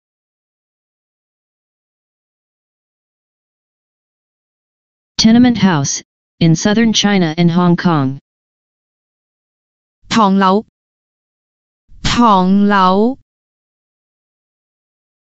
"TENEMENT HOUSE (IN SOUTHERN CHINA AND HONG KONG)" in Cantonese (唐樓) - Flashcard
[tong4 lau2] "TENEMENT HOUSE (IN SOUTHERN CHINA AND HONG KONG)" in Cantonese (唐樓) - Flashcard. Additional information about the word: tenement house (esp. in southern China and Hong Kong).